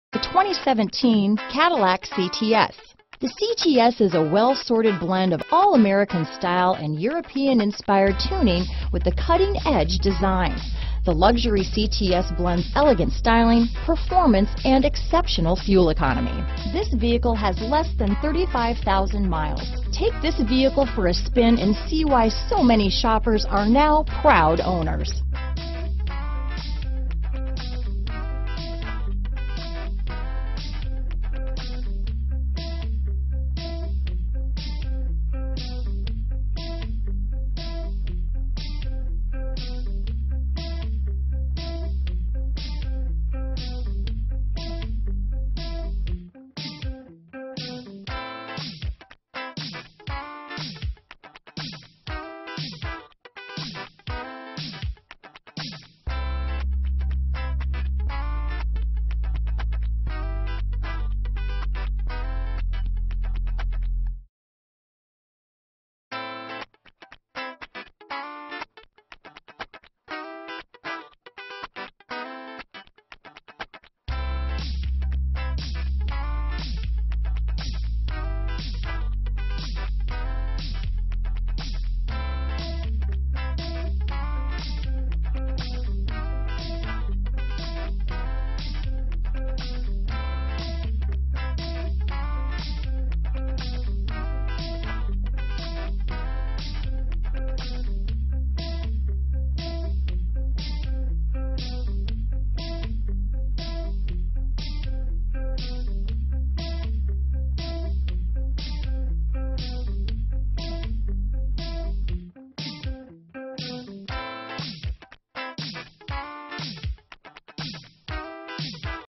2017 Cadillac CTS Westborough, Worcester, Framingham, Acton, Fitchburg, MA 80714A
Crystal White Tricoat Used 2017 Cadillac CTS available in Westborough, Massachusetts at Colonial CDJR. Servicing the Worcester, Framingham, Acton, Fitchburg, MA area.
2017 Cadillac CTS 3.6L Luxury - Stock#: 80714A - VIN#: 1G6AX5SS9H0174238

Colonial CDJR
24 Coolidge St